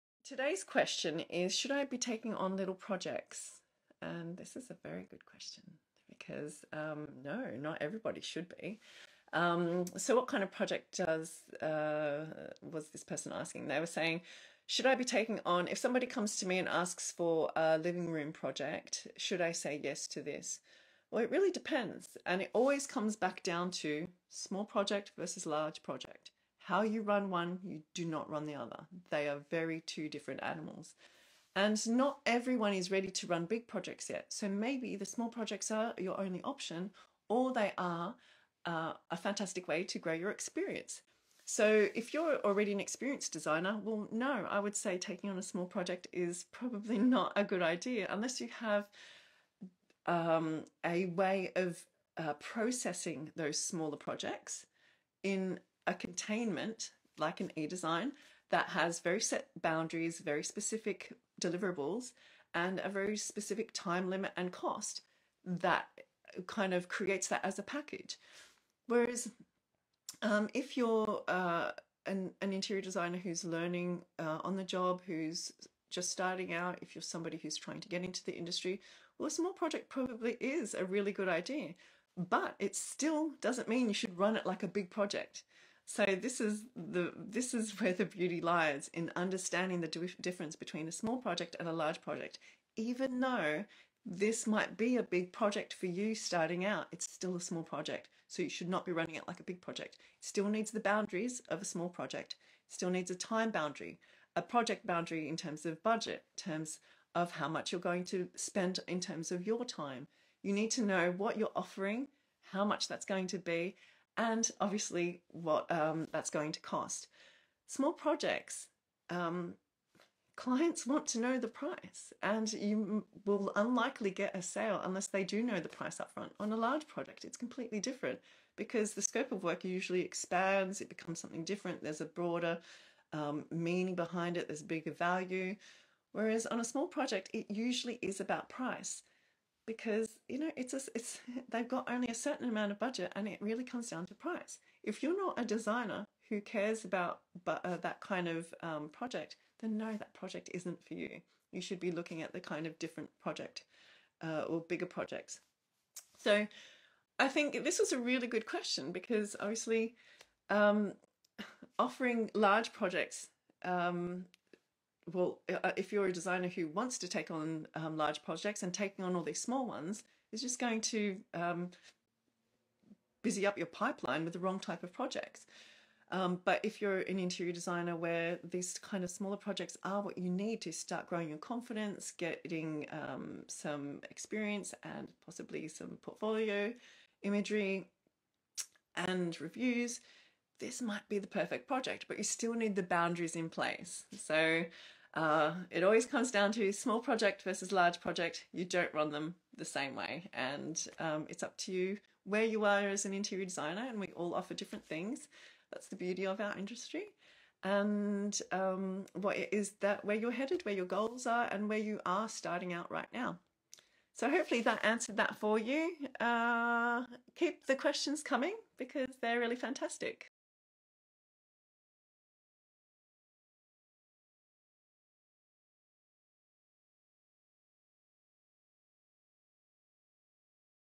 Should You Take On Small Interior Design Projects?
If you're a new designer, small projects can be a fantastic way to gain experience, grow your portfolio, and build a client base.

If you're a more experienced designer, then taking on small projects is likely a bad idea! If you're filling your pipeline with small projects, you won't have the bandwidth to take on the projects you actually want. So, in this case, taking on small projects is more likely to be a pain than helping you achieve your desired business goals.

I always say that you can't run a small project like a big one (and you shouldn't run a big project like a small one). Both have their value, but you need to know the differences and use them to your advantage, depending on where you are in your career as a designer.

If you enjoyed this Q&A, join me live on Instagram, or send me a question you want answered.